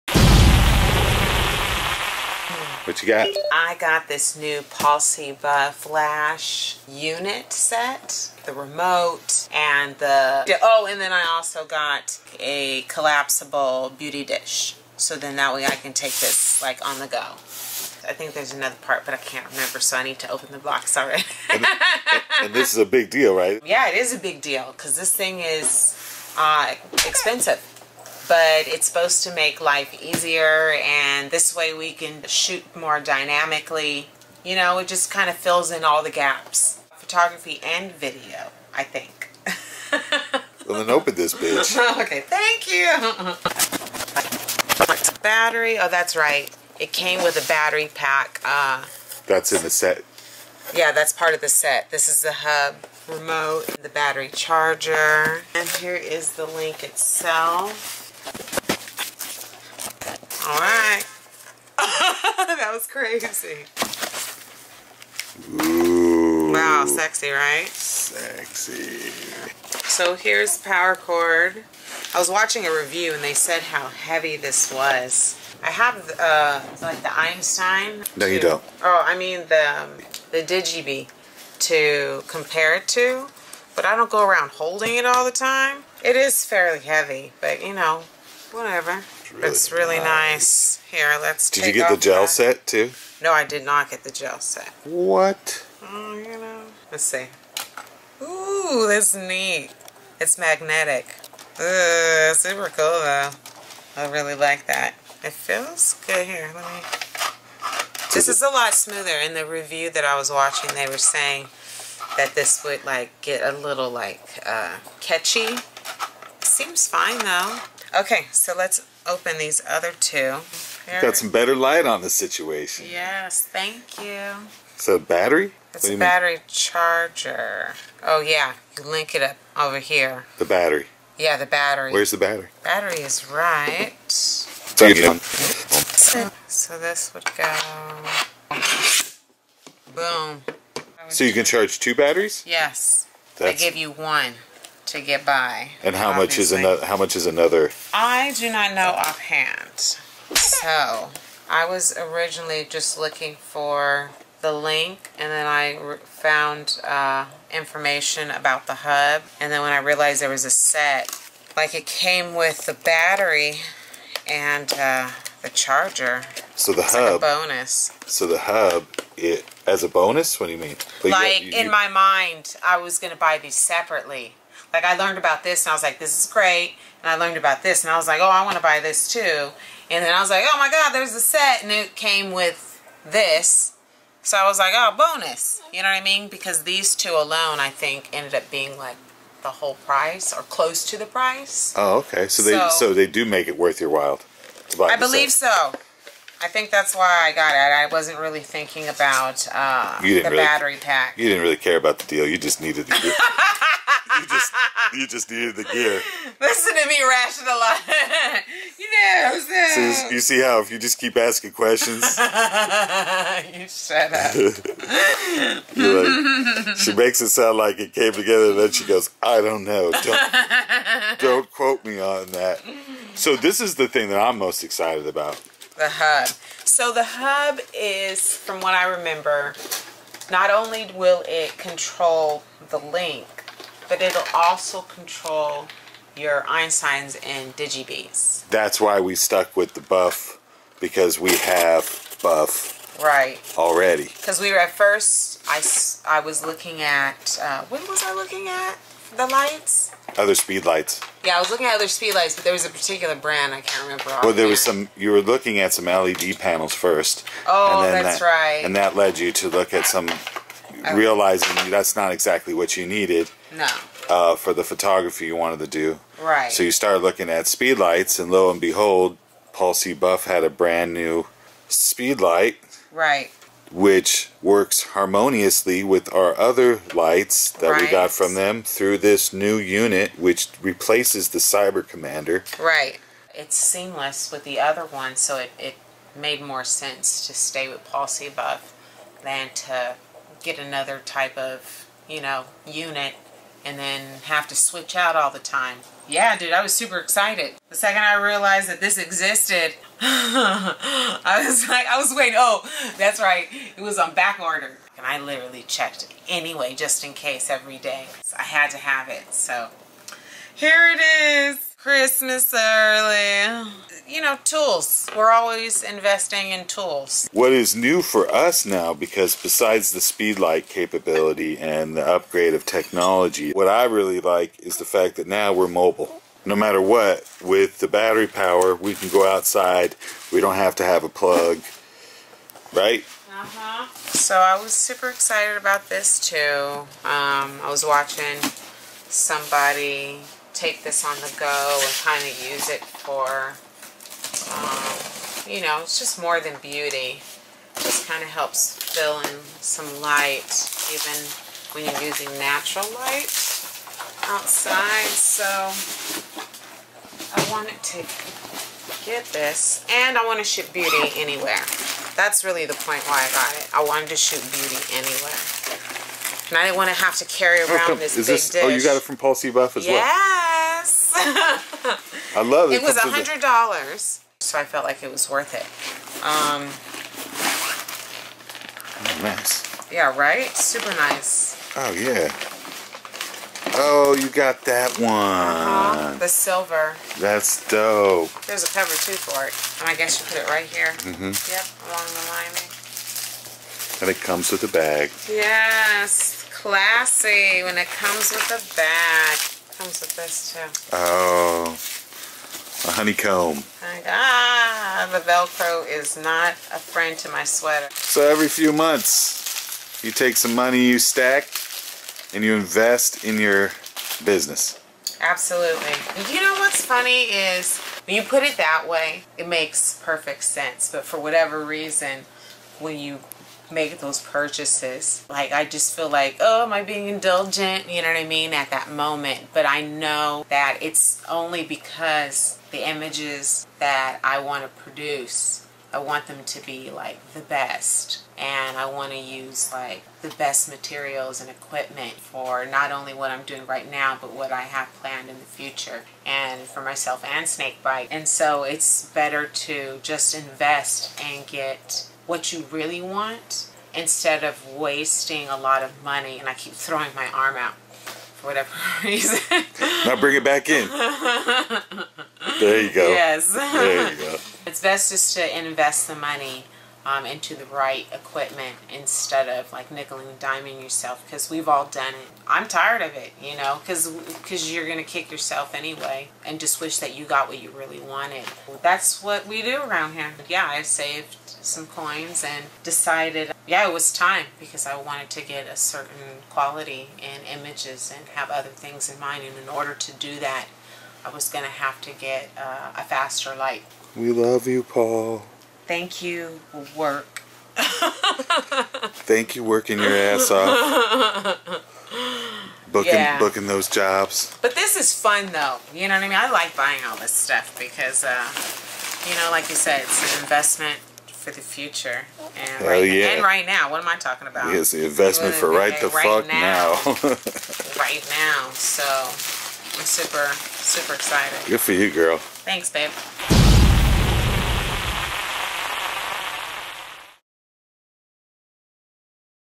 PAUL C. BUFF LINK UNBOXING! Shedding some MUCH NEEDED light on things.
Breastesses around the Globe are shedding some much needed light on the LINK that overshadows them all!

Paul C Buff has been a great illuminating contributor to this cause. But don’t feel like you’re left in the shadow of giants. Anyone can plug in and see for themselves.

Investing in their Link Package was the obvious move for us. It seemed like an inside of the box resource for our studio that we had to unpack.

Samee pulled the trigger and made it happen!

And BOOM, just like that, everything became that much clearer! Thats what she does!

YOU CAN SUPPORT OUR FLOW NOW! Thankful for this new app developed by youtubers who care about new and smaller channels. Gratitude